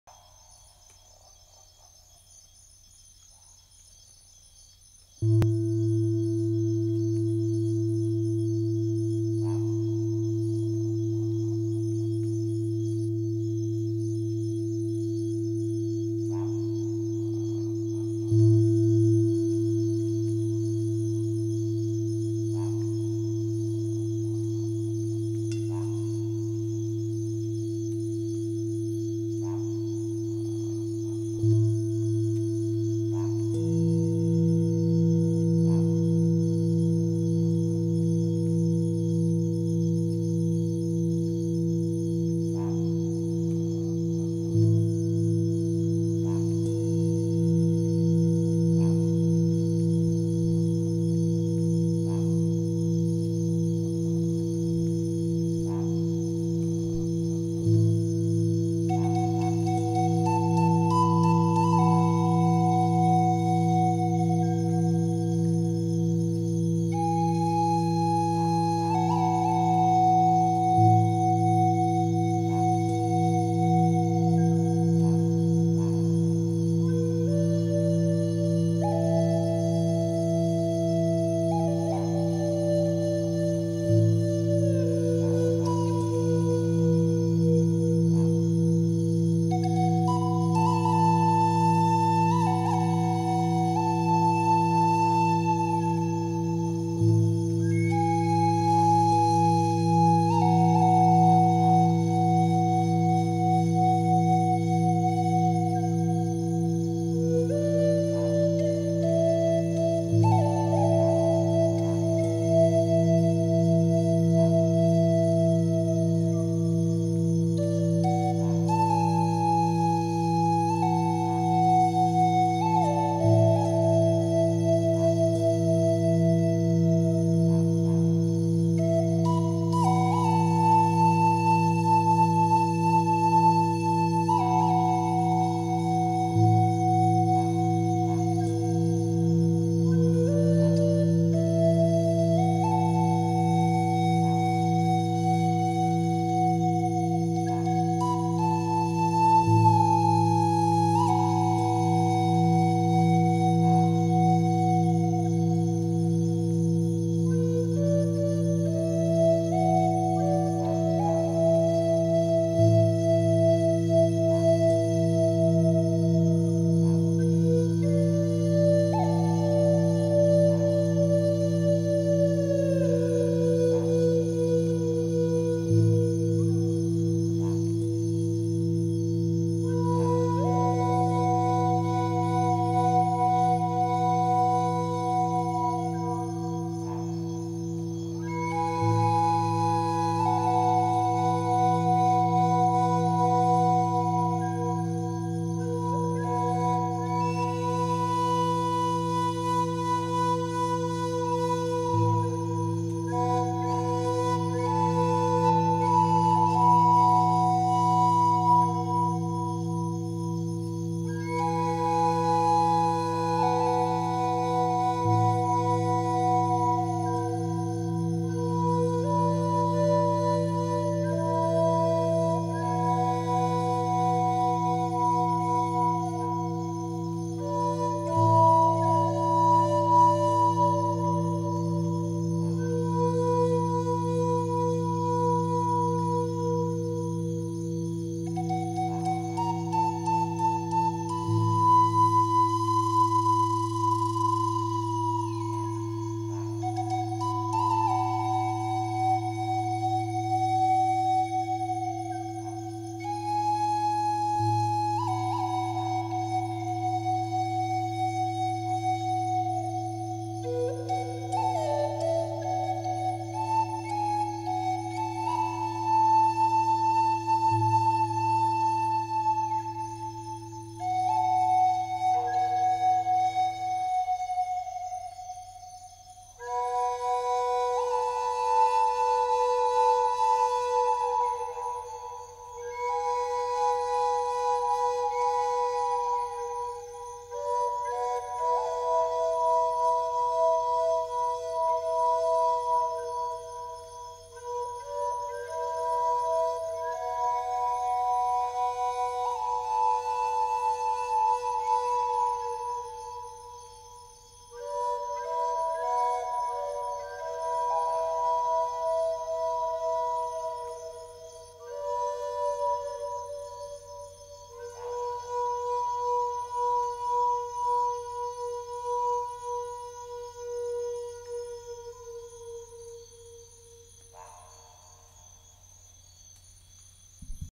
RC 505 looping Tibetan Bowls and Flute
Take this moment to close your eyes, come back to your breath and let the sounds carry you home.

Using RC 505 to loop up some jungle samples, Tibetan Healing Bowls and Bamboo Flute